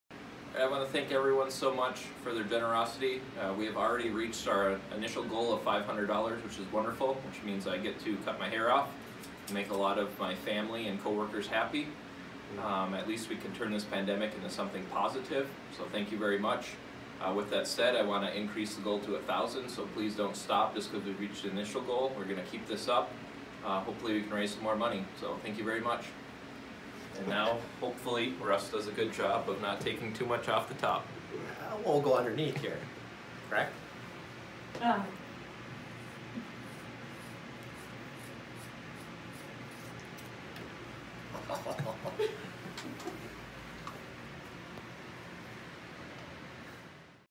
Matt Lutz Pandemic Haircut Fundraiser!
Want to join me in supporting a good cause? When the pandemic started and barber shops/salons were forced to close, I said I was going to grow out my hair until the pandemic was over. I never thought it would last over a year and in retrospect I wish I hadn’t made that comment. Now that the mask mandate has been removed some are signaling it as the end of the pandemic. I thought some good should come from the growing of my hair for past 15 months and decided why not raise money for the Dickinson Area Community Foundation (DACF). Please consider donating to the DACF knowing your donation is “For Good. For Ever.”

Facebook pays all the processing fees for you, so 100% of your donation goes directly to DACF.